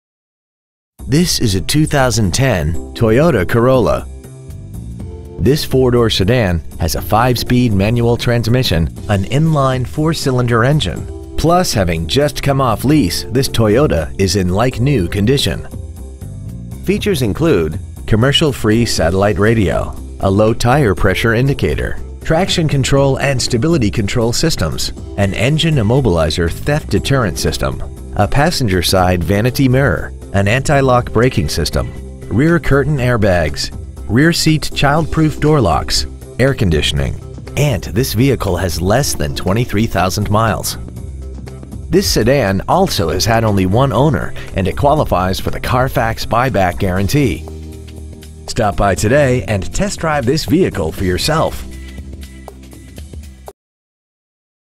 Preowned 2010 Toyota Corolla Boca Raton FL
Year : 2010
 Make : Toyota
 Model : Corolla
 Engine : Gas I4 1.8L/110
 Trans . : Manual
 Exterior : Desert Sand Mica
 Miles : 22,740
 Interior : Bisque
 Stock : 43628

 Preowned 2010 Toyota Corolla Lake Worth FL